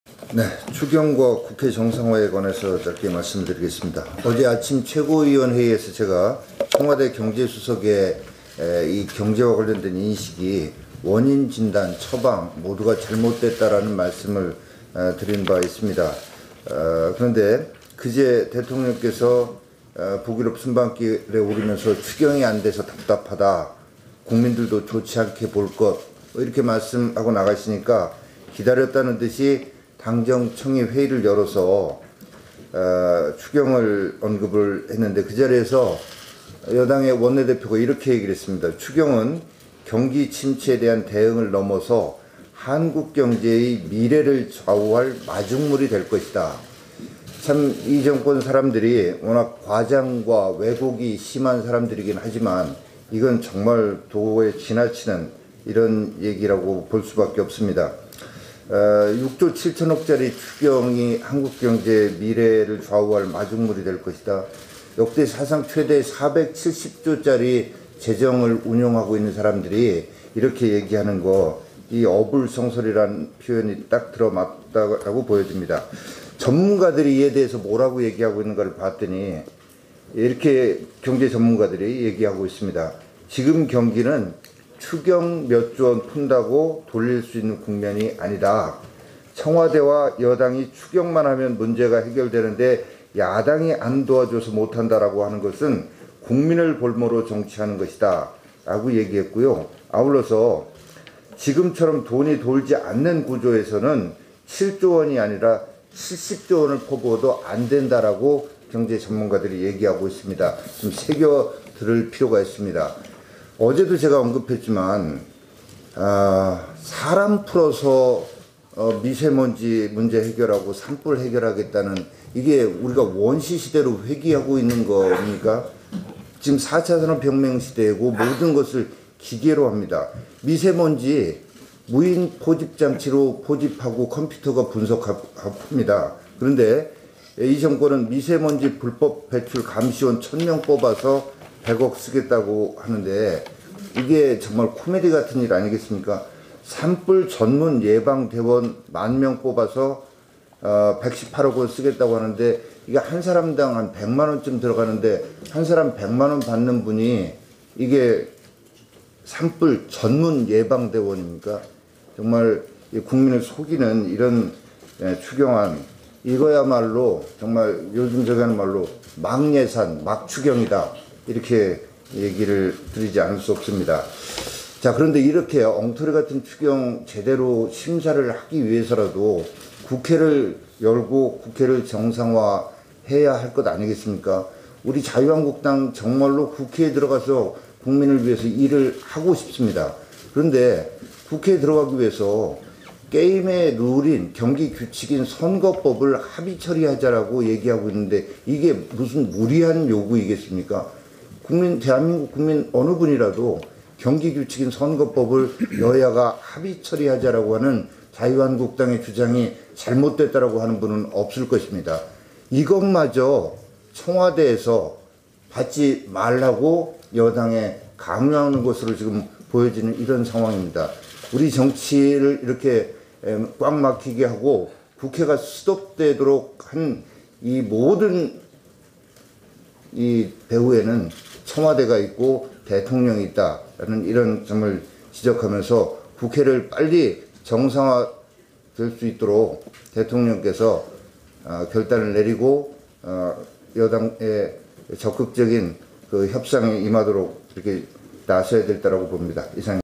자유한국당 정용기, "국회 스톱 배후는 대통령, 막예산 막추경 엉터리 추경"
정용기 자유한국당 원내대책회의(풀발언)